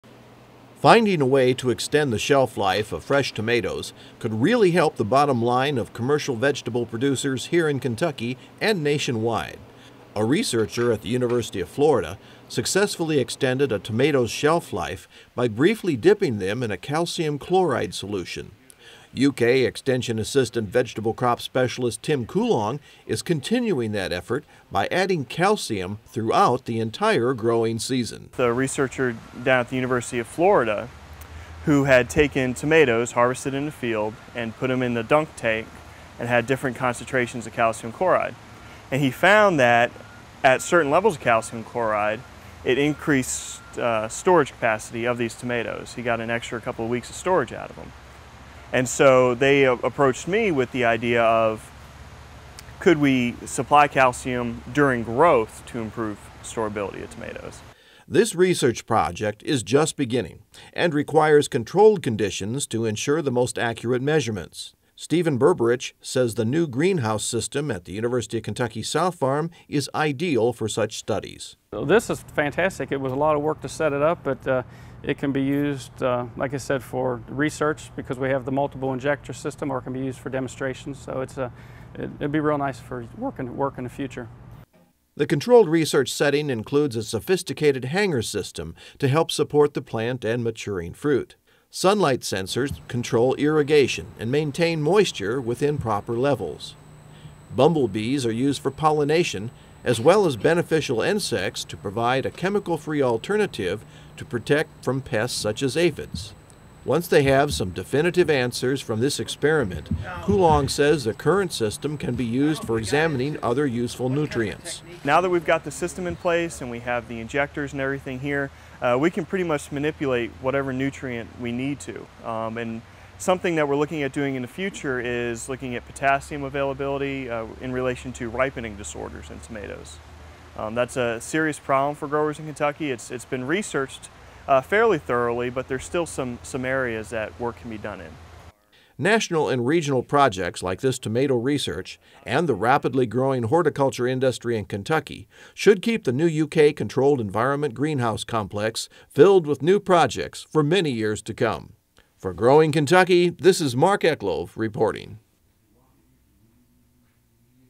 Tomato Research - Growing Kentucky, December 2009
Extending the shelf-life of tomatoes is the goal of research being conducted at the University of Kentucky, as researchers look for a way to make these healthy fruits last longer.